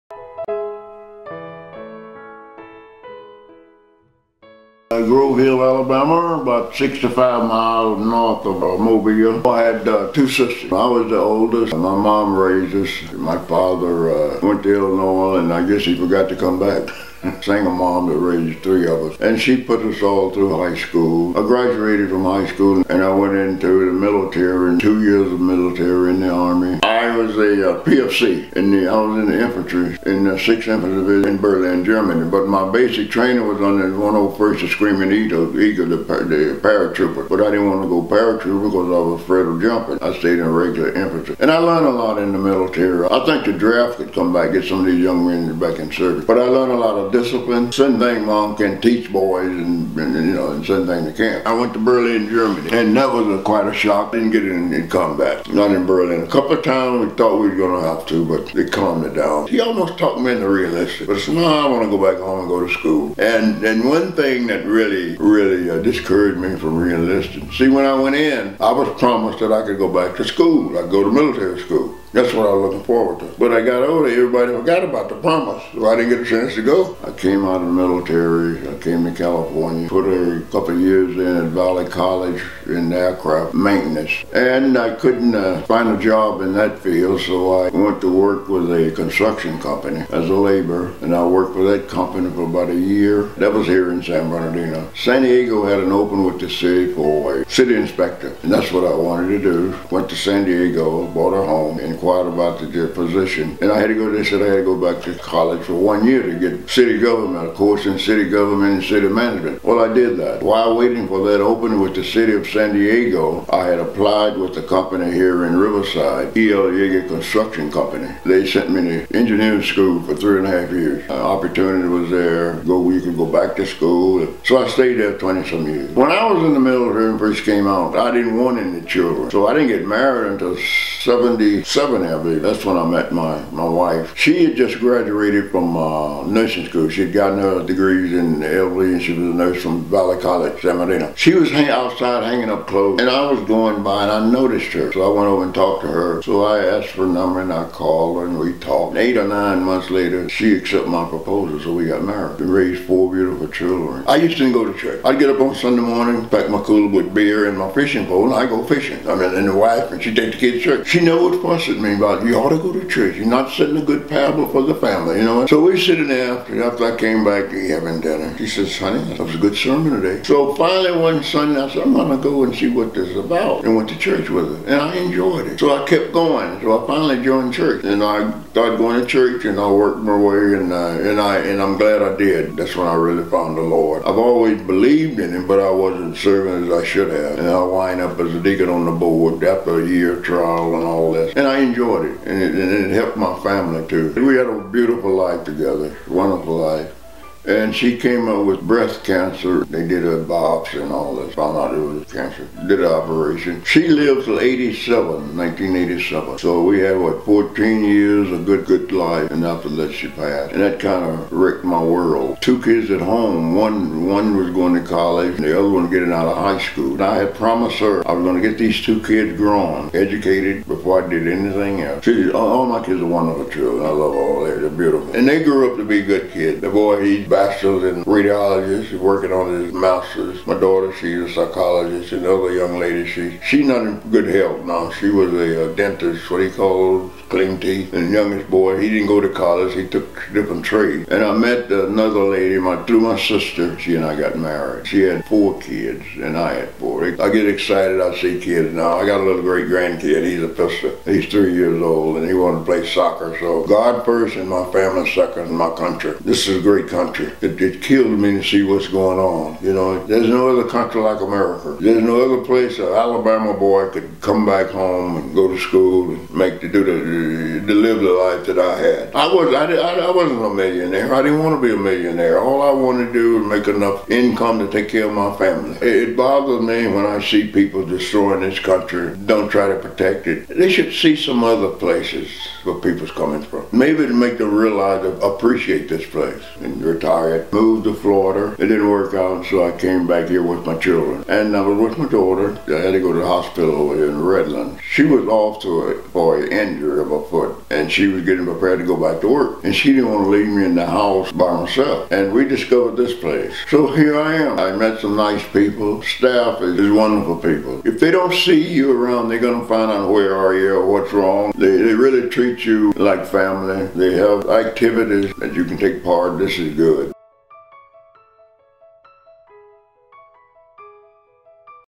Resident Spotlight: Charles A.
Meet Charles, one of the residents at Renaissance Villages. This man is full of passion for his kids and his country. He discusses his time in the Army and his love of learning. Watch his story to learn more.